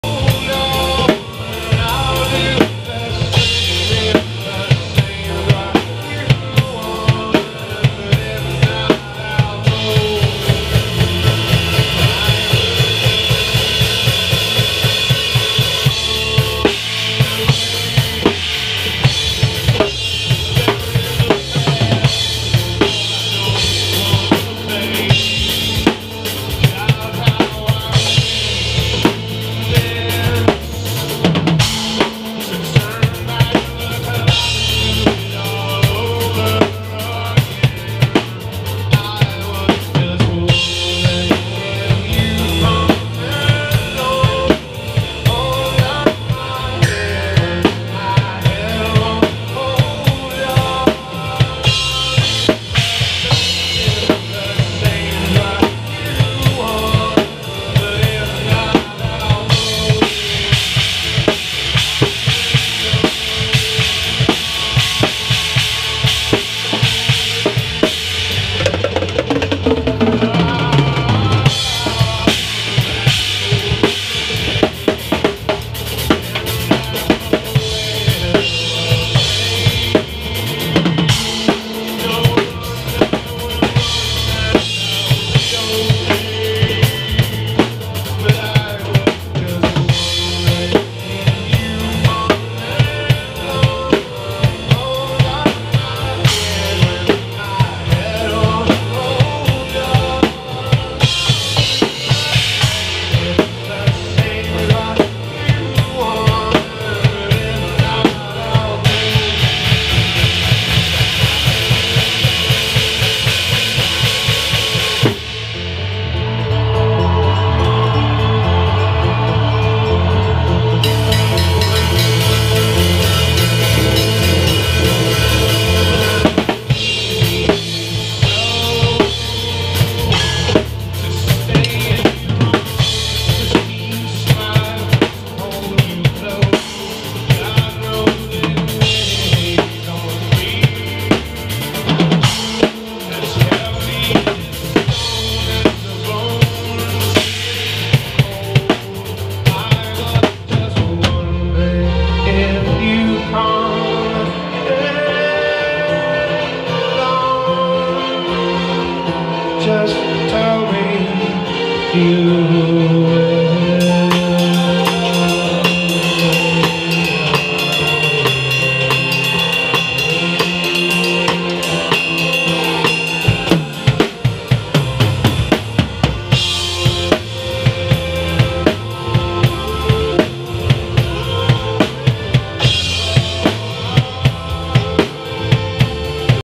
Part of Riley Steelface 11yrs old practicing Dave Matthews Band DMB live.
Part of Riley Steelface 11yrs old playing Dave Matthews Band DMB live.